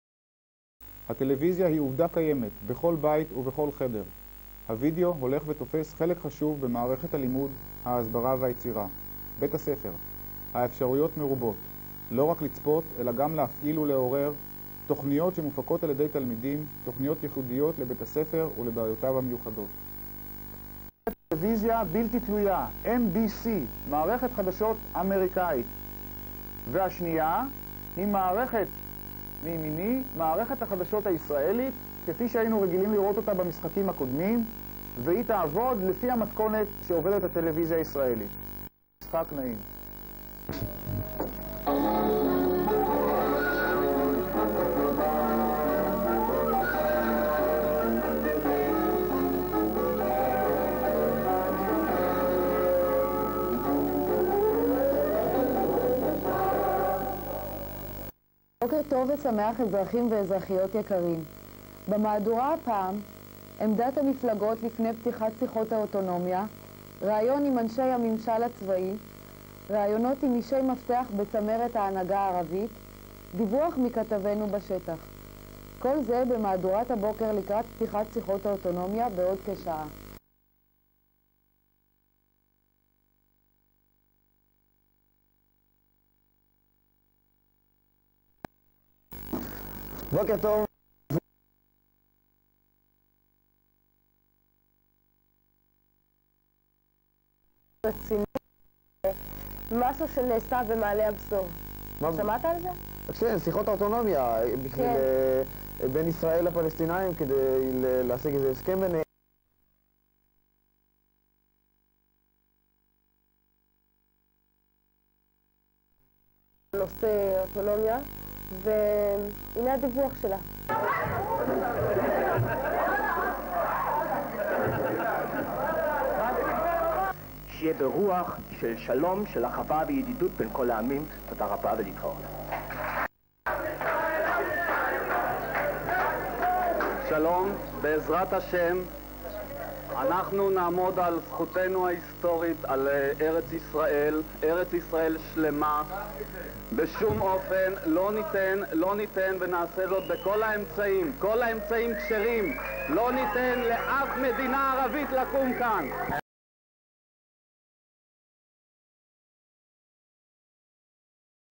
multicamera live 1982
Live broadcast with high school students in Maale Ha'bsor school in 1982